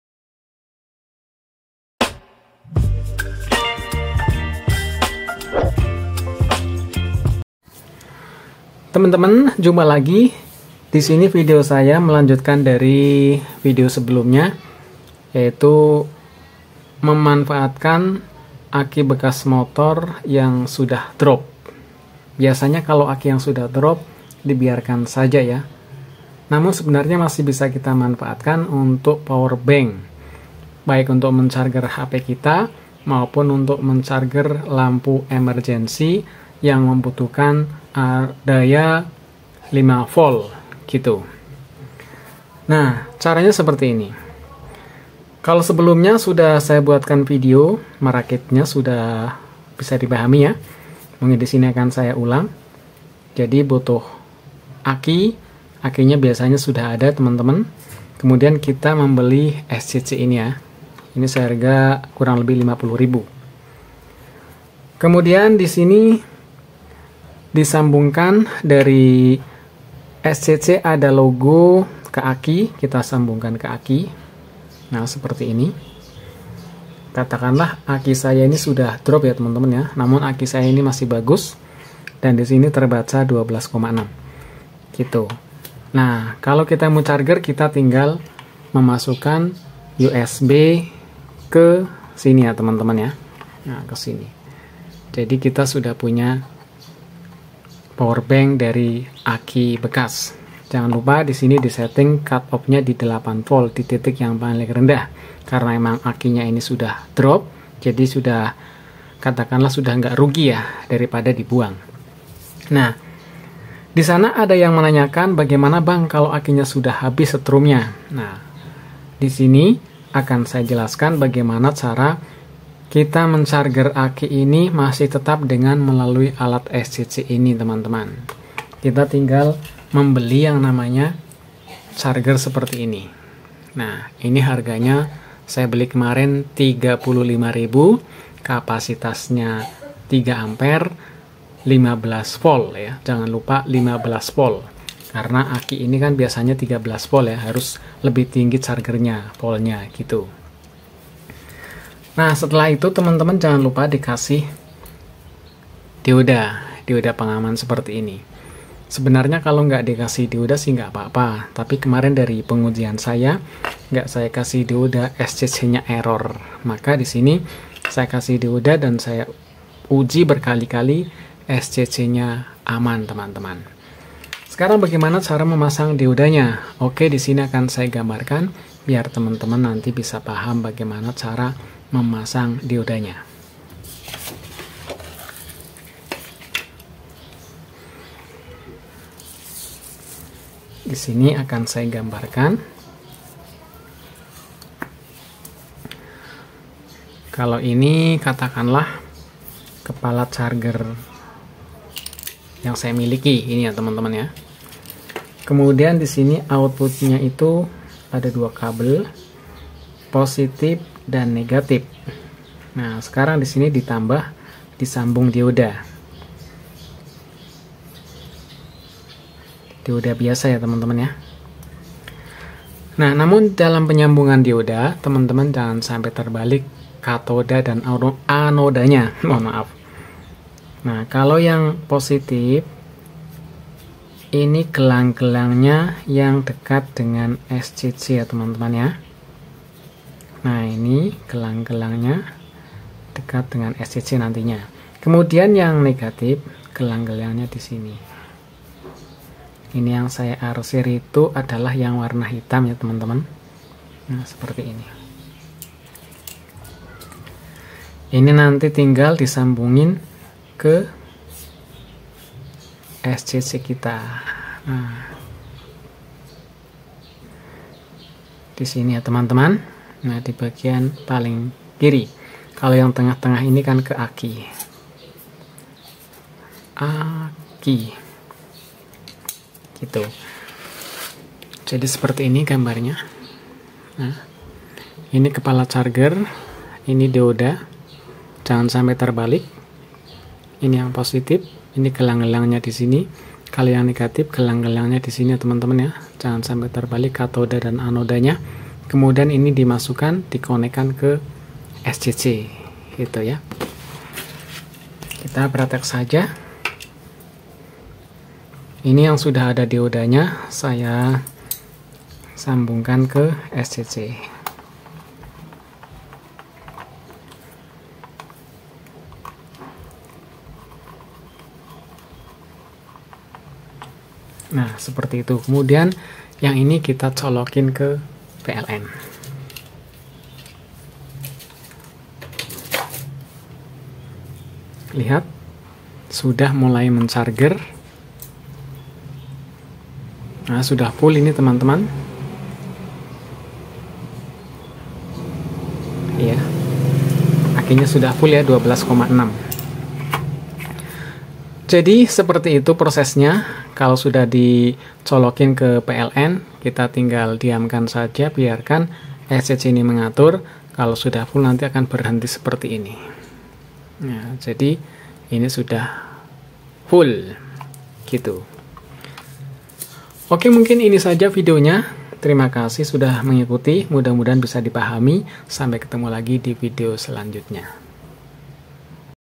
Cara charger aki bekas motor untuk POWER BANK darurat
Cara charger aki bekas motor untuk POWER BANK darurat,menggunakan chrager aki belanja online dengan harga 35.000,- dapat kapasitas 3a 15volt Dc.